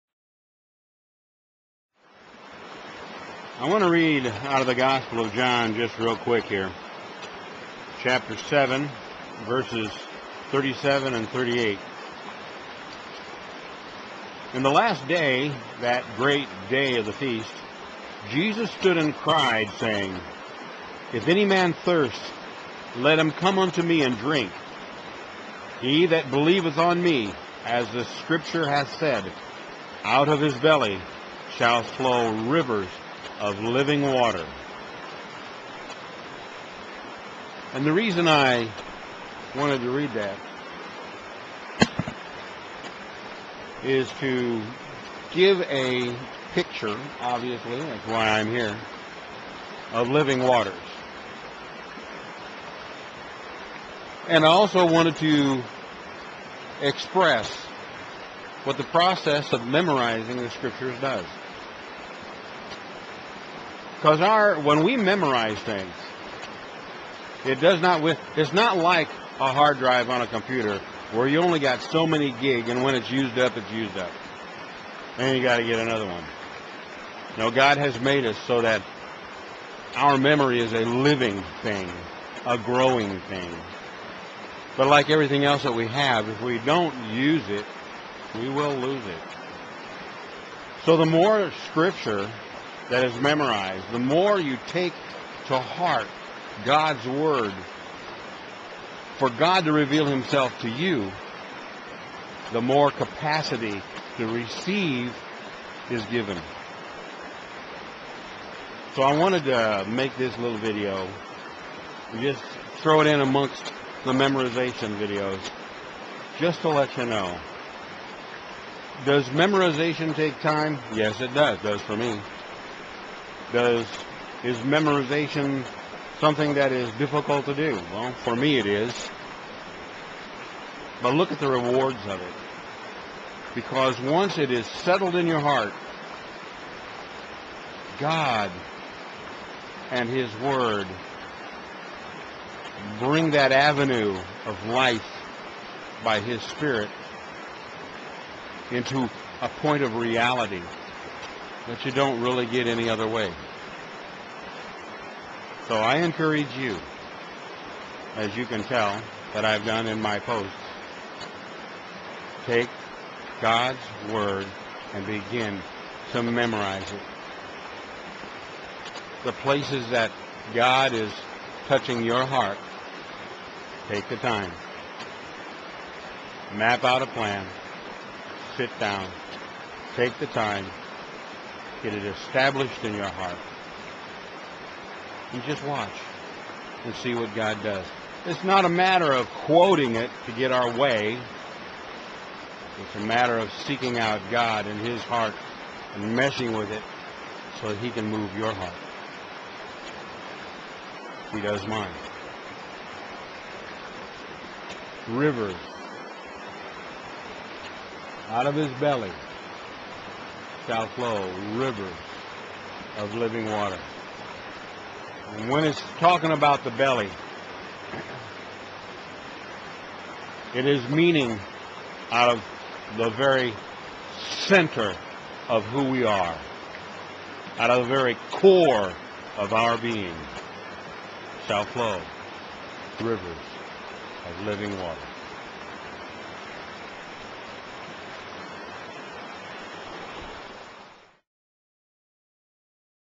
Why I Memorize My Bible
In a culture that is so waste conscience it is time to stop wasting the precious memory that God has given us.